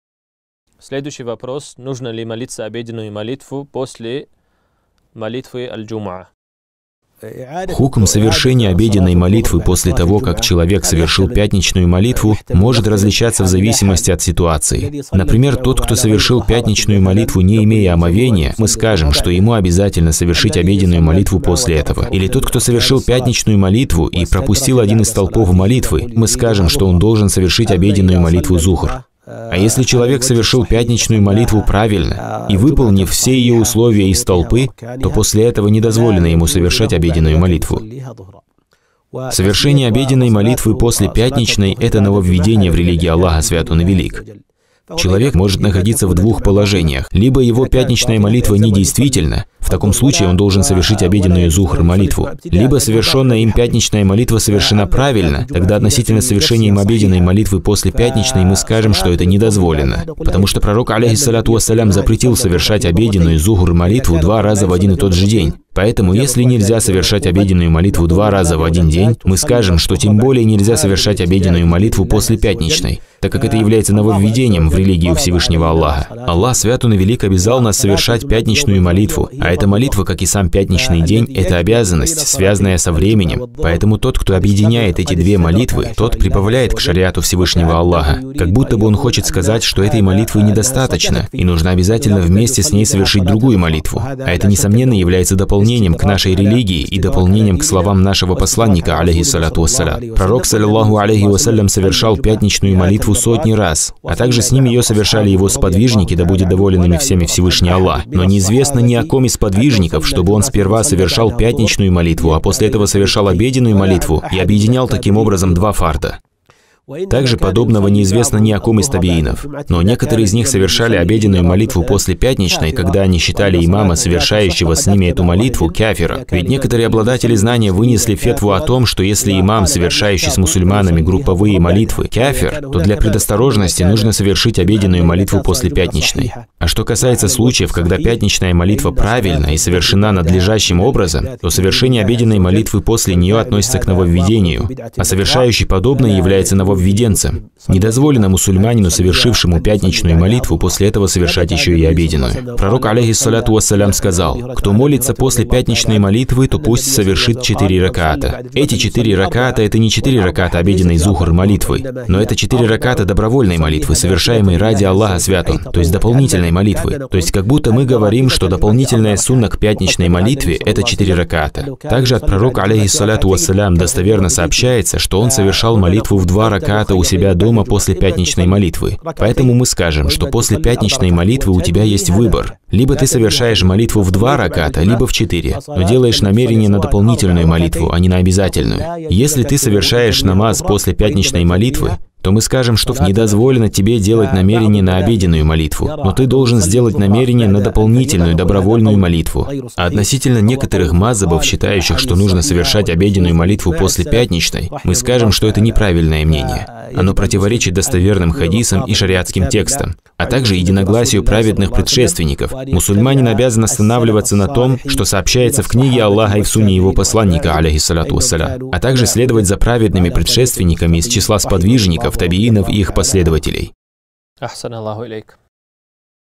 Совершать обеденный намаз после пятничного нововведение? | Шейх Халид аль-Фулейдж
Отрывок из ответа шейха:

Совершение обеденной молитвы после пятничной – это нововведение в религии Аллаха, Свят Он и Велик.

Аллах, Свят Он и Велик, обязал нас совершением пятничной молитвы, а эта молитва, как и сам пятничный день – это обязанность, связанная со временем, поэтому тот, кто объединит между совершением этих двух молитв, тот дополняет шариат Аллаха, Свят Он. Как будто бы он хочет сказать, что этой молитвы недостаточно и нужно обязательно вместе с ней совершить другую молитву. А это, несомненно, является дополнением к нашей религии и дополнением к словам нашего посланника ﷺ. Пророк ﷺ совершал пятничную молитву сотни раз, а также с ним ее совершали его сподвижники, да будет доволен ими Всевышний Аллах, но не известно ни от одного из сподвижников, чтобы он сперва совершал пятничную молитву, а после этого совершал обеденную молитву, чтобы объединить между двумя фардами. Также подобного не известно ни от одного из таби’инов.

Но некоторые из них совершали обеденную молитву после пятничной, только тогда, когда они считали имама, совершающего с ними эту молитву, кафиром, например, и что его молитва не засчитывается. Ведь некоторые обладатели знания вынесли фетву о том, что, если имам, совершающий с мусульманами групповые молитвы, кафир, то для предосторожности нужно совершить обеденную молитву после пятничной. Это касается случаев, когда существует подобная проблема. А что касается случаев, когда пятничная молитва достоверна и совершена надлежащим образом, то совершение обеденной молитвы после нее относится к нововведению в религии Аллаха, Свят Он и Велик, а совершающий подобное, попадает в порицаемое нововведение. Не дозволено мусульманину, совершившему пятничную молитву, после этого совершать еще и обеденную.

рузман